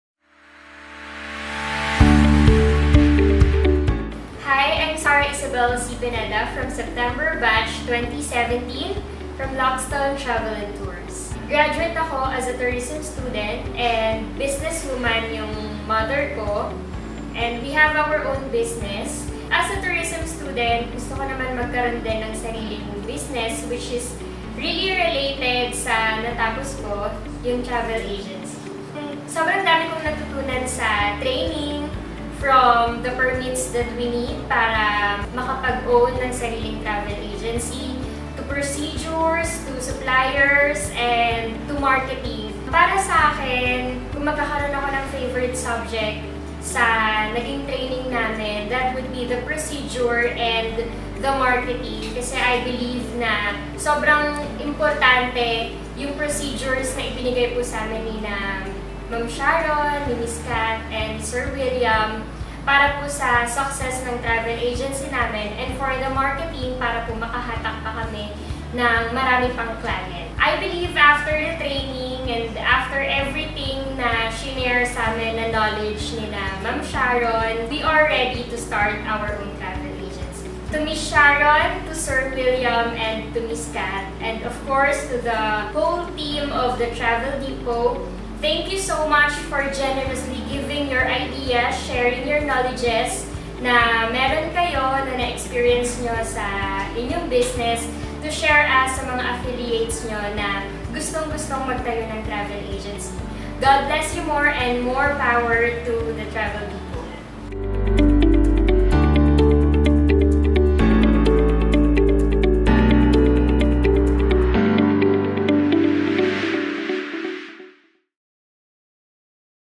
They Chose They Spoke: Isabelle Pineda
Who would’ve have thought that from a young age she already know her passion and what to pursue in life, which is to travel and have a business? That’s how Isabelle Pineda of September Batch 2017 turned her travel entrepreneurial dreams into reality!
We couldn’t be happier knowing that you are fulfilling your dreams little by little. The Travel Depot is always here to support your endeavors in life. More breakthroughs to come and be an epitome to other young entrepreneurs like you!